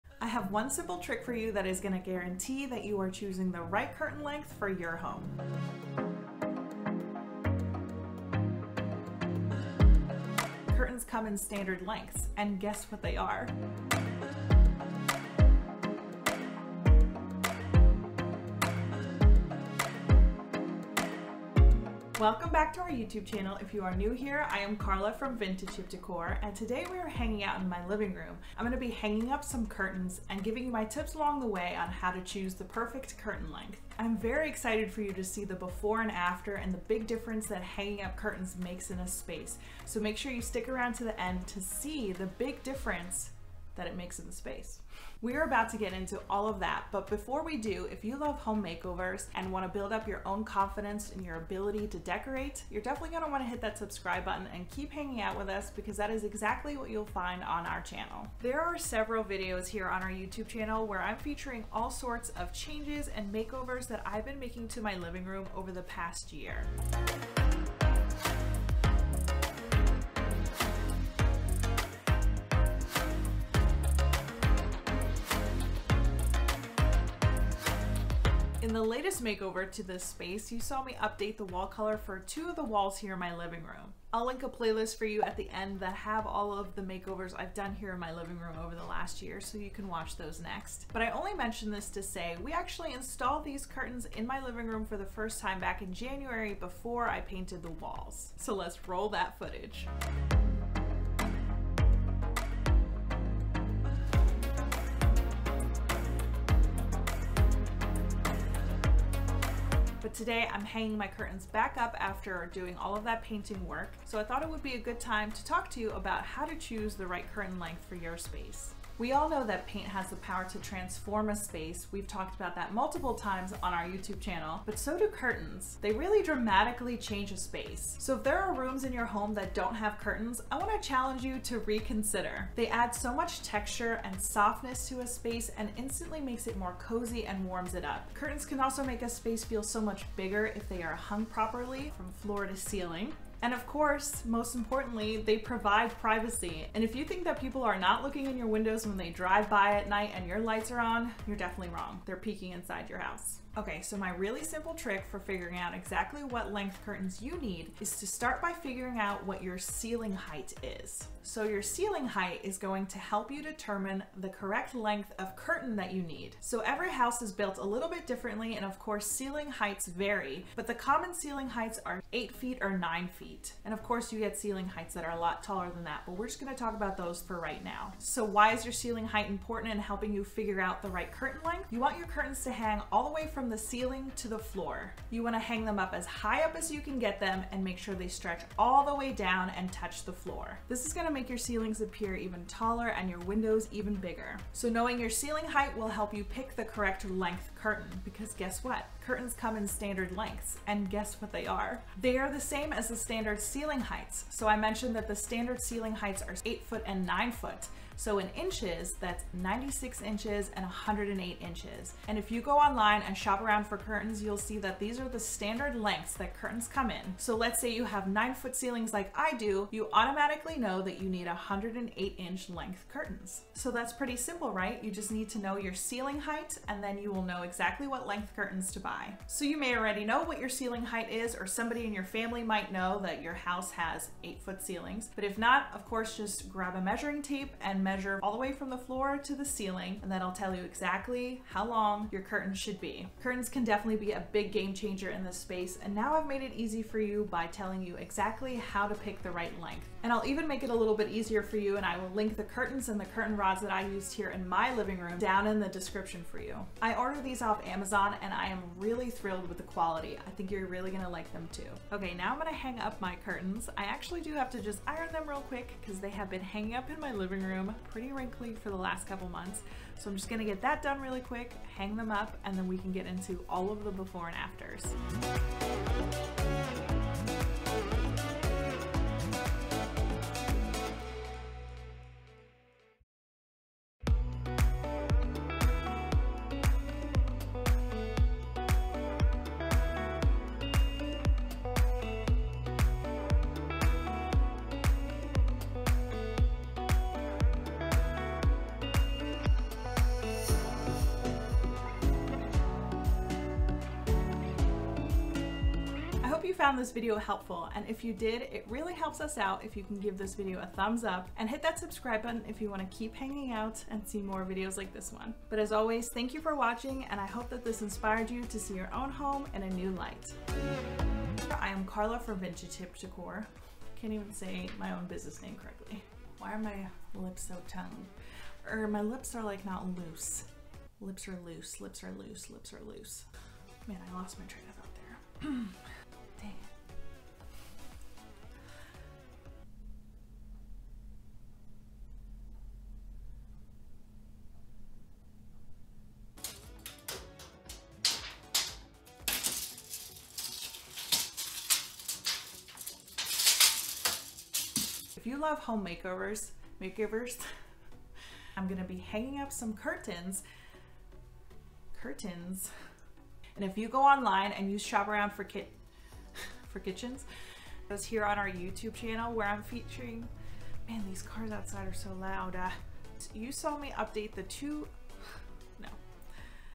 How To Choose A CURTAIN LENGTH and Size
Today we want to share how to Choose a Curtain Length and the importance of window drapery size. Regarding the size of curtains or drape panels, the length and size you choose are key! We've made many mistakes choosing the proper length, mainly! It has to hang right from nearby the ceiling to kissing the floor. Living room curtains and all other drapery look best this way.

So here is what you need to know before you shop for curtains - watch to the end!

📖 Chapters:
00:00 Choosing the right Curtain Lenght
01:59 Curtains make a dramatic impact
04:43 Before and after...